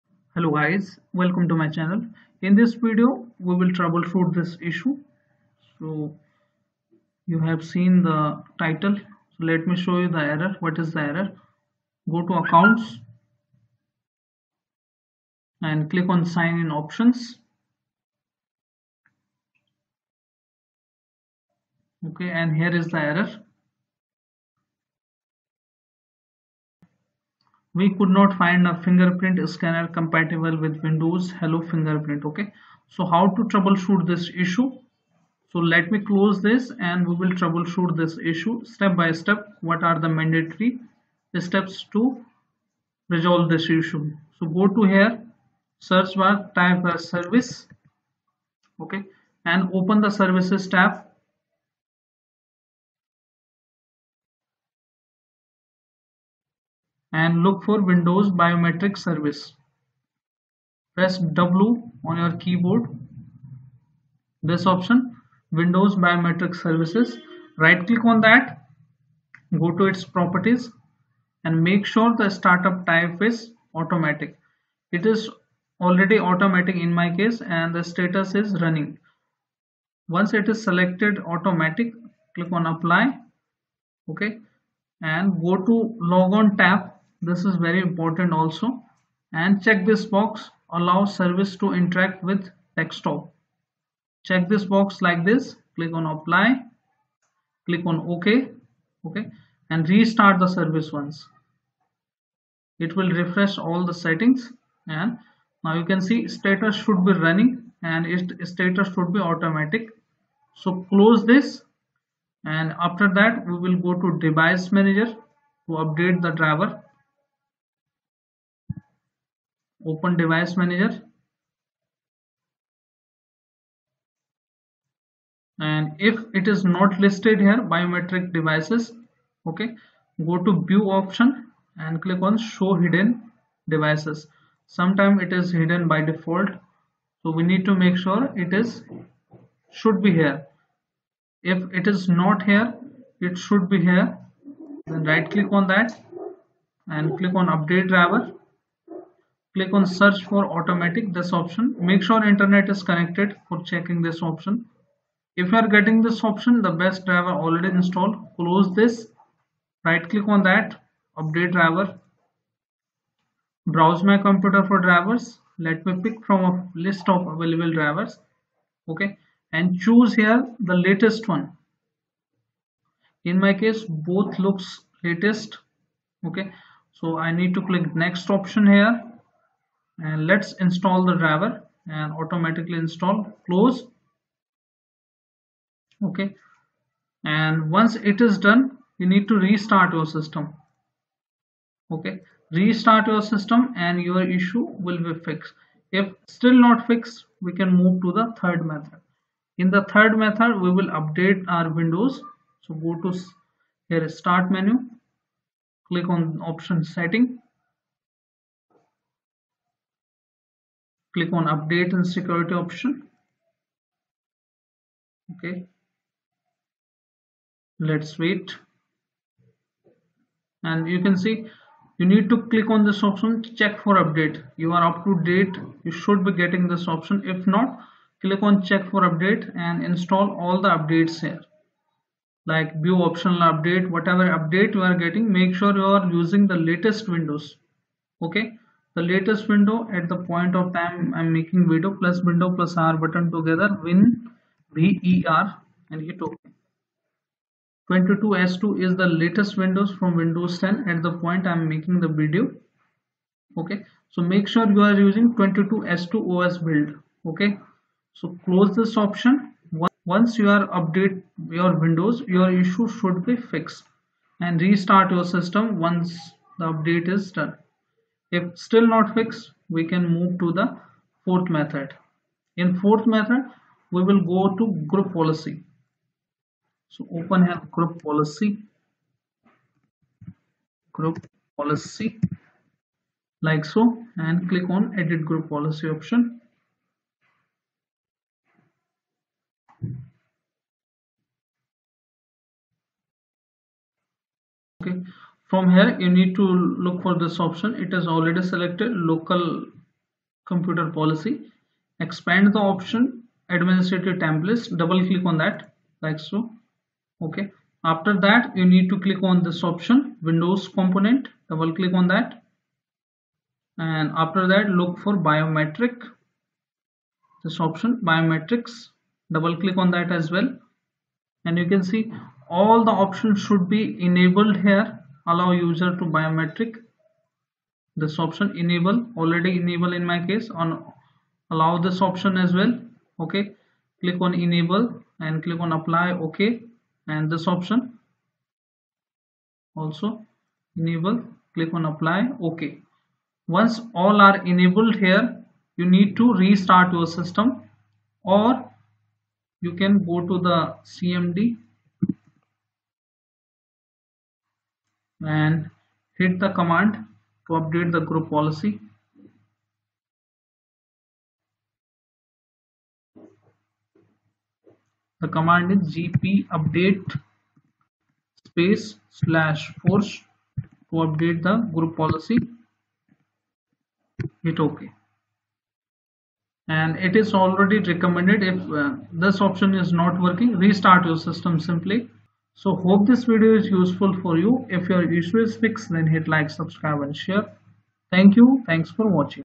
We couldn’t find a fingerprint scanner compatible with Windows Hello Fingerprint In Windows 10/11
Step 1: Look Biometric service

Step 2: Update Biometric Driver

Step 3: Update Windows to Latest
winver

Step 4: Enable fingerprint scanner By Group Policy

Local computer policy\administrative templets\windows components\Biometric
gpudate /force